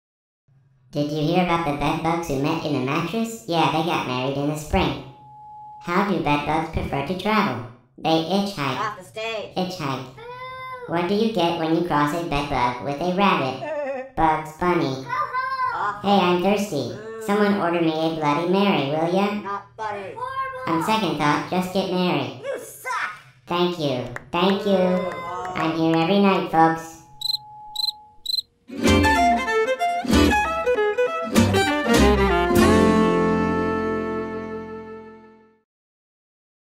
Bed Bug Stand Up Comic
Just a funny little video from animator Cable Hardin of a bed bug stand up comedian.

Yes, there is a funny side to pest control sometimes!